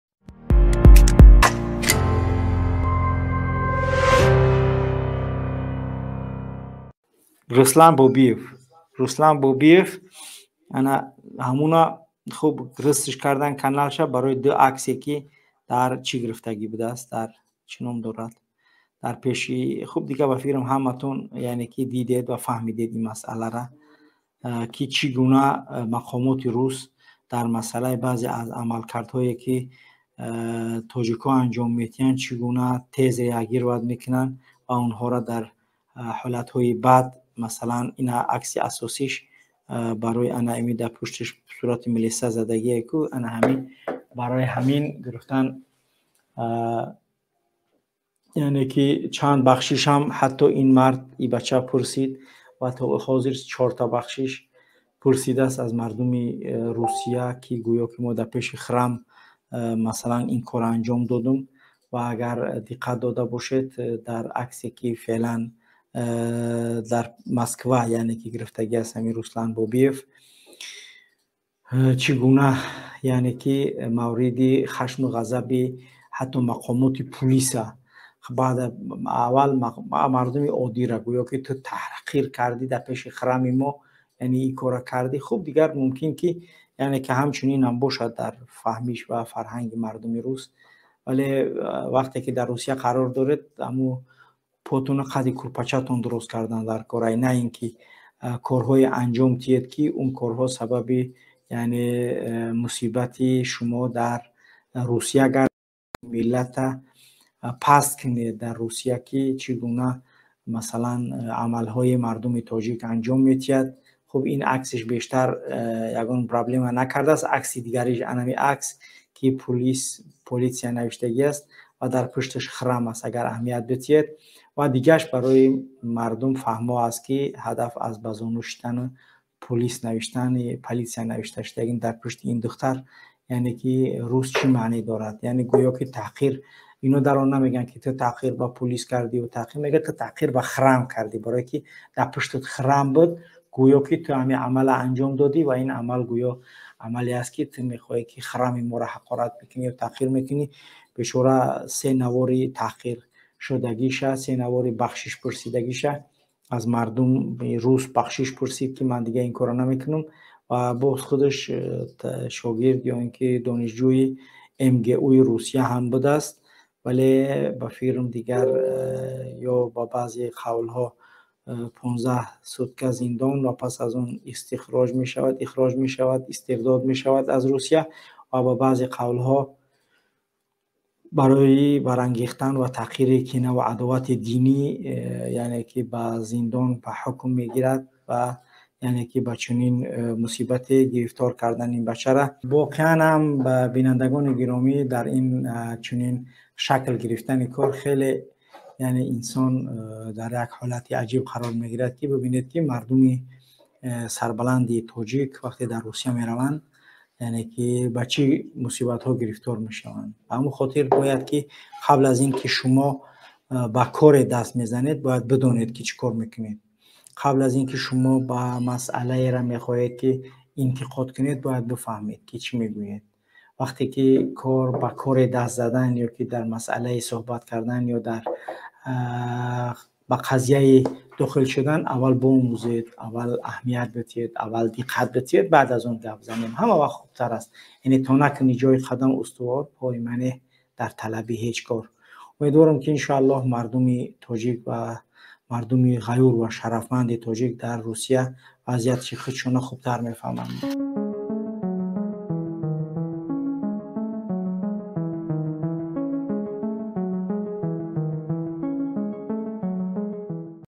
Мурочиати Мухаммадикболи Садриддин ба мухочирон барои Руслан Бобиев || хулосаи гап
салом бинандагони азиз
дар ин видео Мухаммадикболи Садриддин др бораи Руслан Бобиев  Сухбат мекунад
 ва мурочиат ба мухочиро
Muhammadiqboli Sadriddin
tegs:
дурбин , durbin, Мухаммадикболи Садриддин , Минбари мухочир